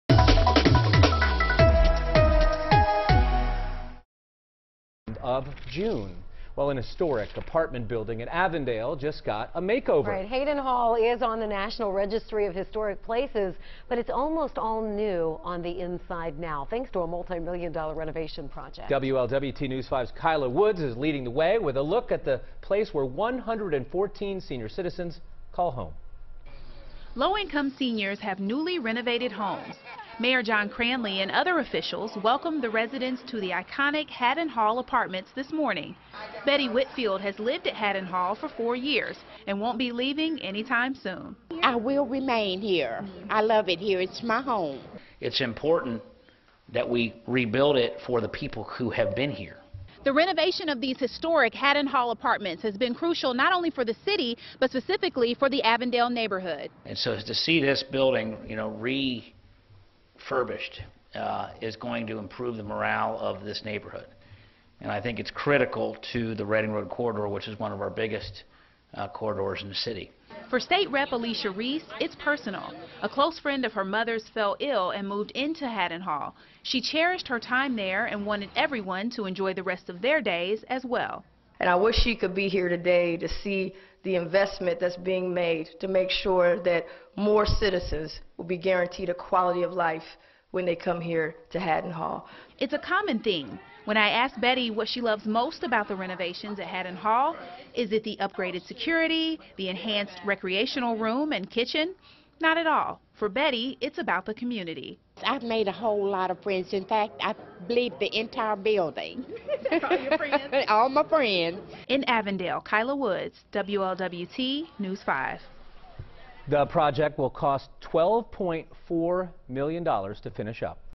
Haddon Hall apartments to be restored
Mayor John Cranley and other city officials joined Wallick Communities on Monday to cut the ribbon and mark the restoration of the iconic Haddon Hall Apartments. Hall currently serves over 100 low income seniors.